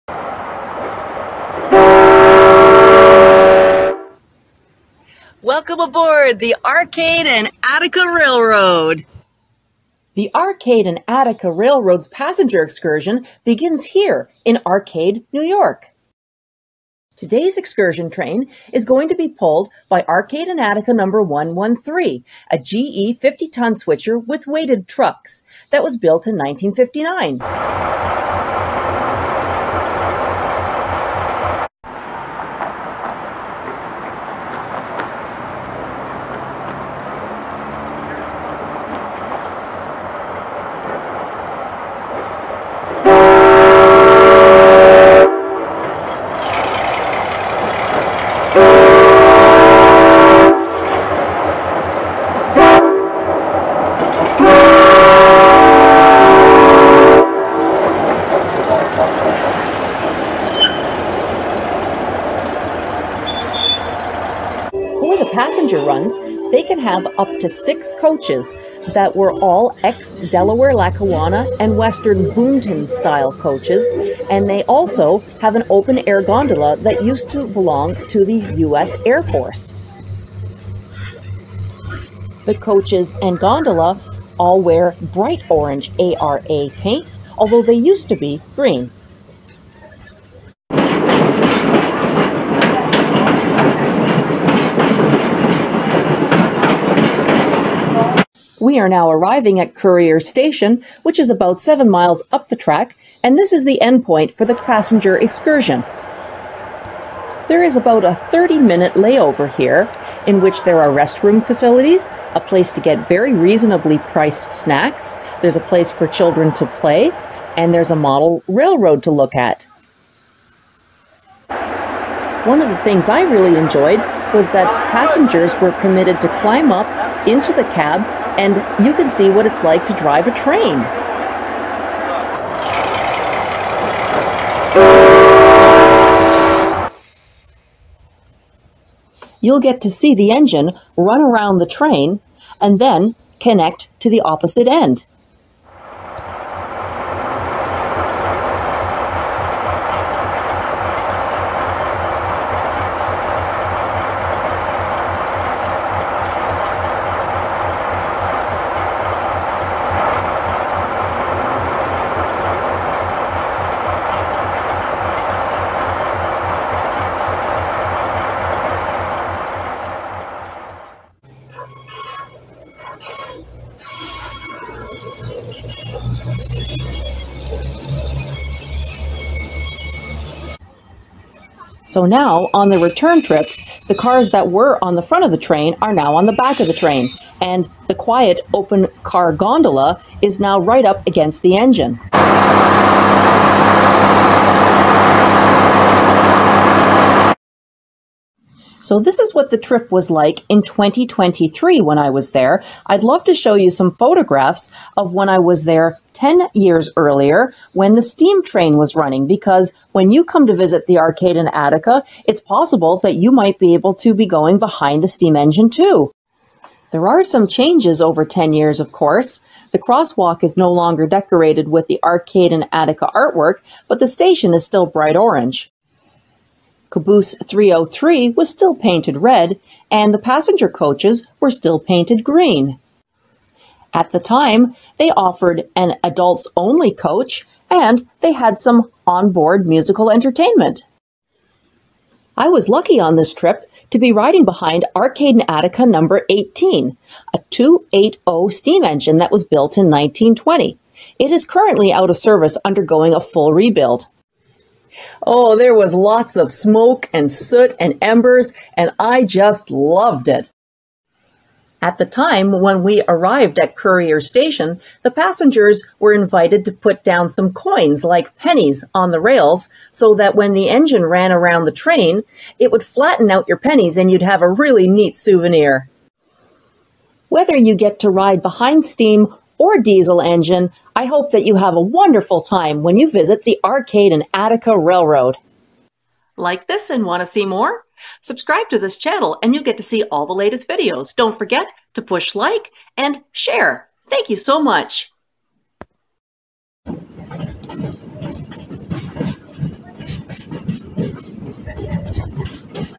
Riding the ARCADE & ATTICA Excursion Train in New York State behind Diesel AND Steam
Rode the Arcade and Attica in 2013 behind their steam engine
and again in 2023 behind their diesel engine.
Come and see what's in store for you at the ARA!